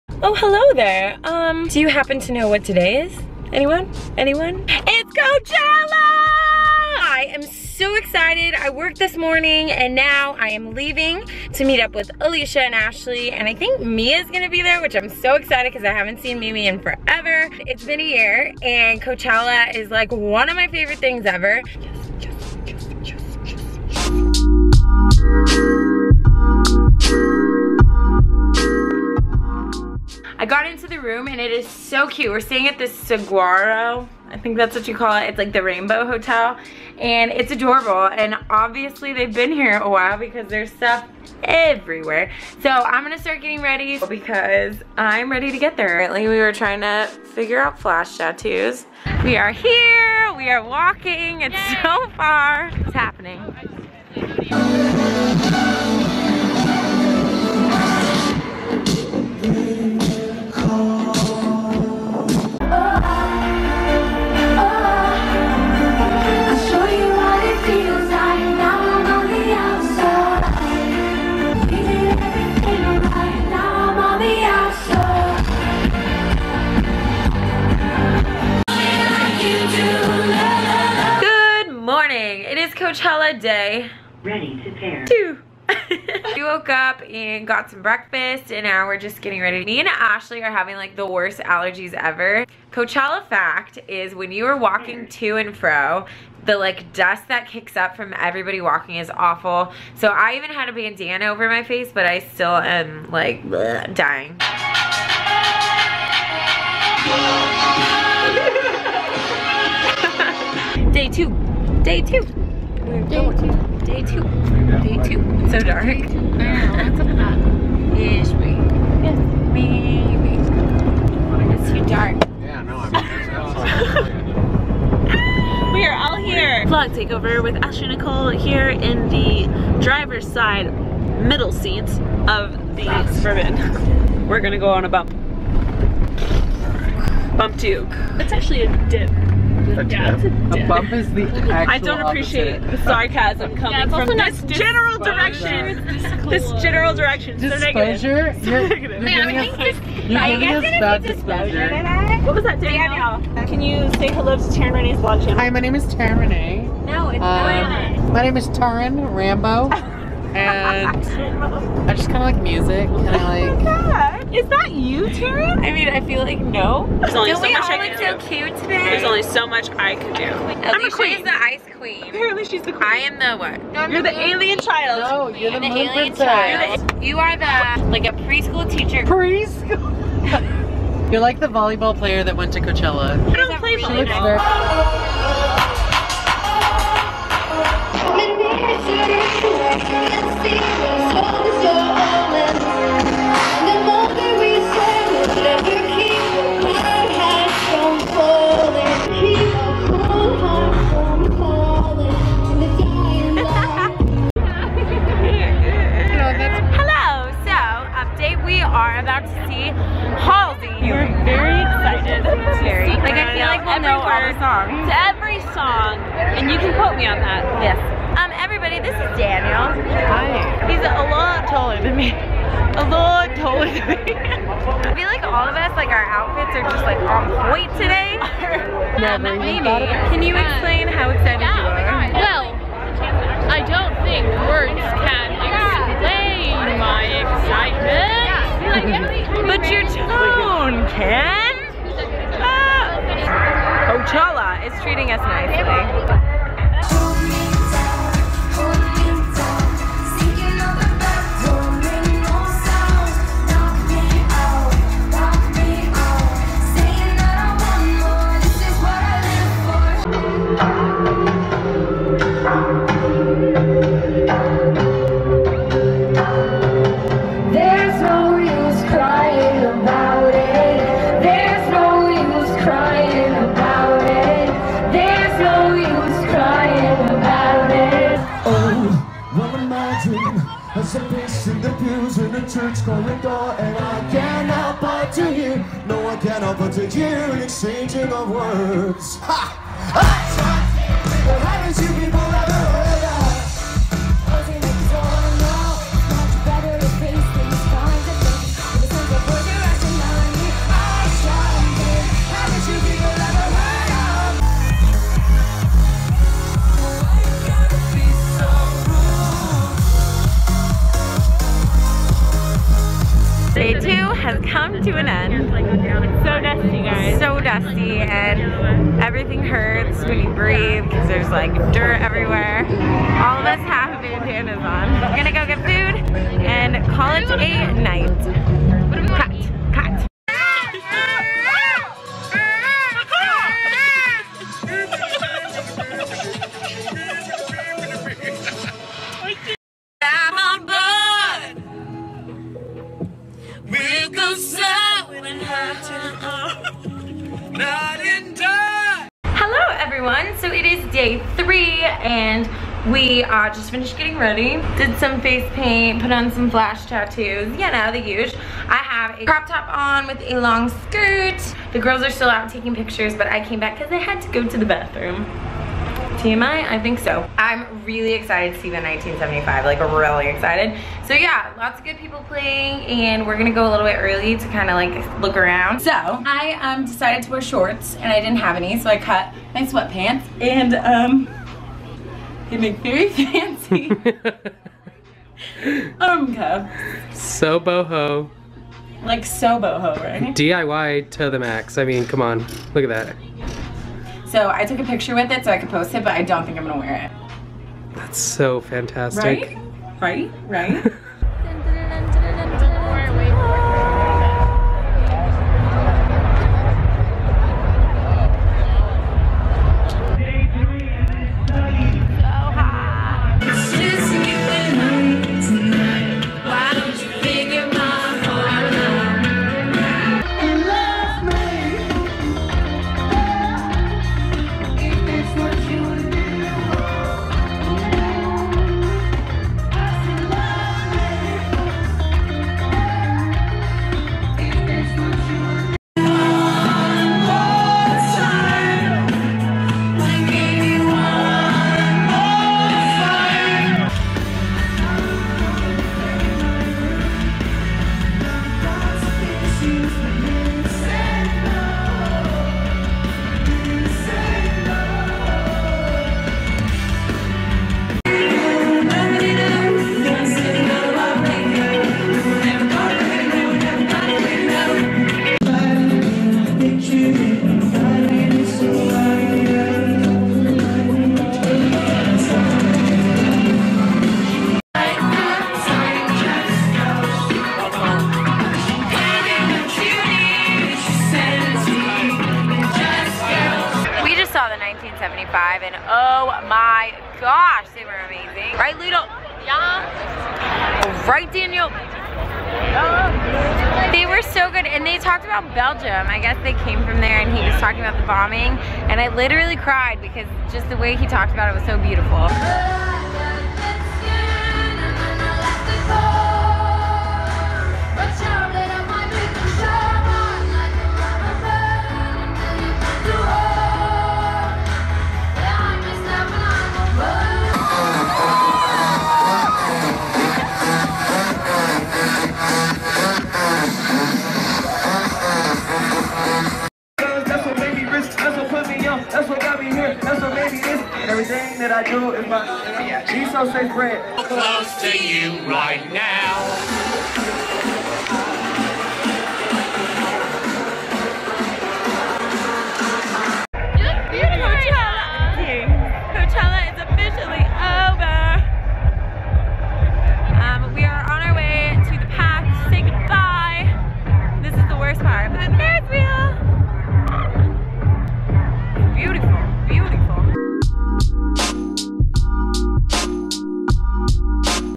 COACHELLA 2016!!!!
Coachella!!! I got to go dance in a field listening so some of my favorite bands with my besties!!! It was magical!